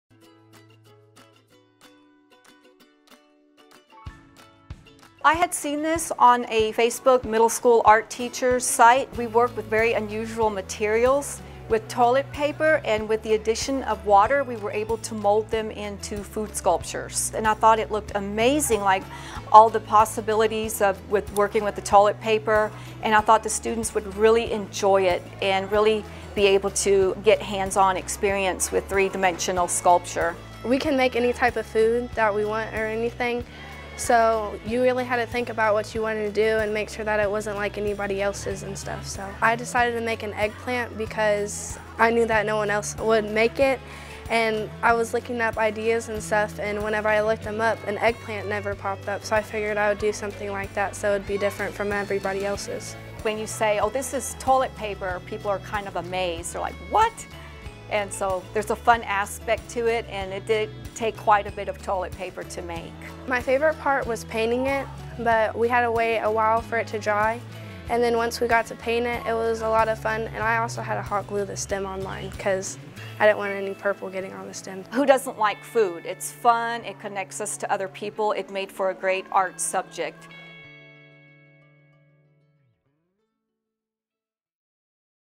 Lawton Public Schools: Toilet Paper Sculpture Art
Students at Eisenhower Middle School flexed their creativity by making food sculptures out of toilet paper. Check out some of their finished pieces, but try not to get hungry along the way!